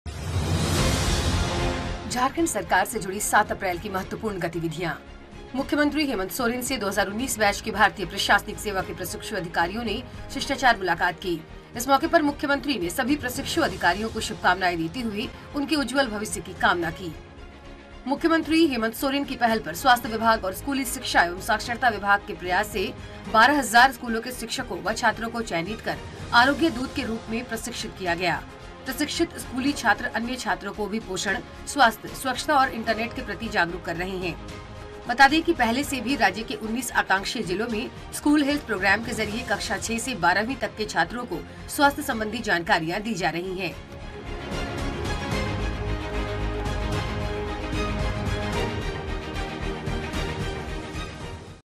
झारखंड सरकार से जुड़ी 7 अप्रैल की महत्वपूर्ण गतिविधियां, जानें खास बातें
Jharkhand 2019 बैच के प्रशिक्षु आई.ए.एस अधिकारियों ने की मुख्यमंत्री हेमंत सोरेन से शिष्टाचार भेंट। दूसरी ओर मुख्यमंत्री हेमंत सोरेन की पहल पर स्वास्थ्य विभाग और स्कूली शिक्षा एवं साक्षरता विभाग के प्रयास से 12 हजार स्कूलों के शिक्षकों व छात्रों को चयनित कर आरोग्य दूत के रूप में प्रशिक्षित किया गया। प्रशिक्षित स्कूली छात्र अन्य छात्रों को भी पोषण, स्वास्थ्य, स्वच्छता और इंटरनेट के प्रति जागरुक कर रहे हैं।